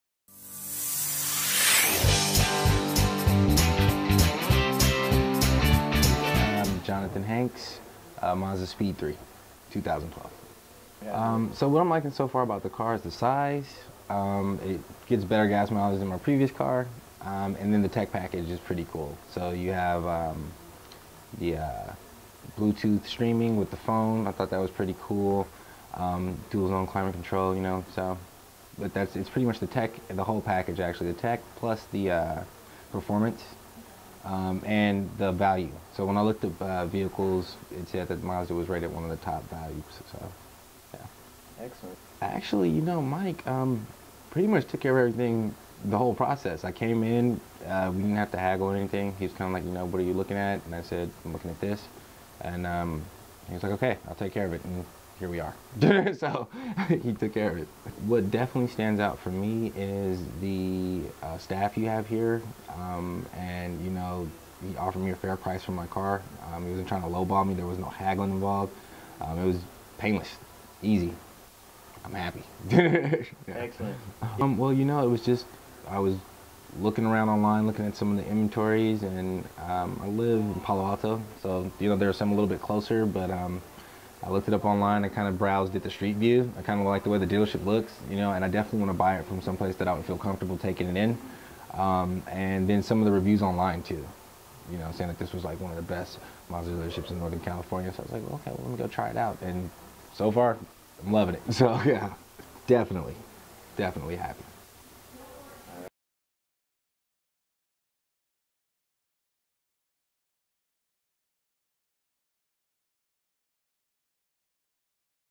2012 Mazda3 - Customer Review | Capitol Mazda | San Jose, CA
This happy car buyer bought his Mazda 3 at Capitol Mazda in San Jose at the Capitol Expressway Auto Mall. The Del Grande Dealer Group wants to make sure you are a happy car buyer, find out why he is leaving Capitol Mazda with his new Mazda as a happy car buyer. He was pleased with the experience of working with his sales associate Mike.